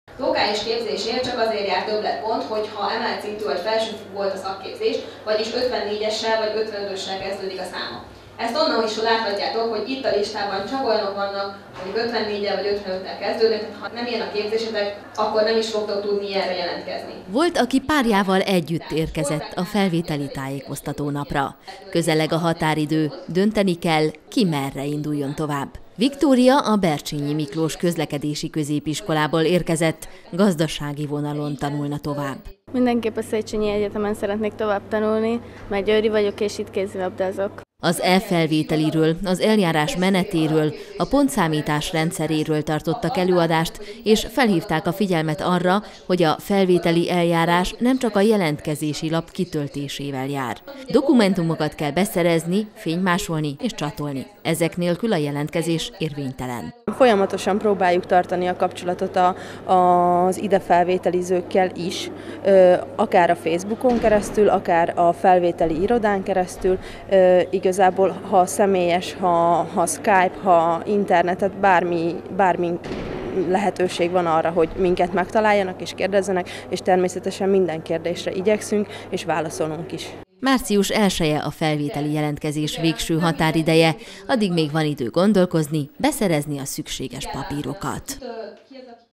Felvételi tájékoztató
Közeleg a felvételi jelentkezés végső határideje. A Széchenyi István Egyetemen, csütörtökön tájékoztató napot tartottak a felvételi eljárás menetéről és a pontszámítás rendszeréről. Az érdeklődők, akik kötetlenül beszélgethettek a campus oktatóival, az új aulában maguk alakították az előadások menetét kérdéseikkel. Március 1-ig kell meghoznia minden felvételizni szándékozónak a döntést: hol tanuljon tovább. Ebben segített a győri egyetem.